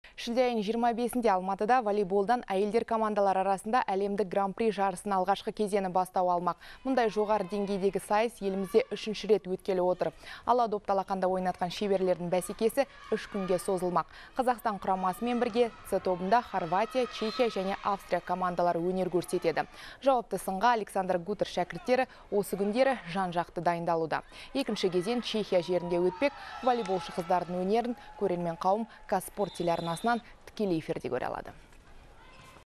Волейболшылар Гран-при жарысына дайындалуда
Мұндай жоғары деңгейдегі сайыс елімізде үшінші рет өткелі отыр. .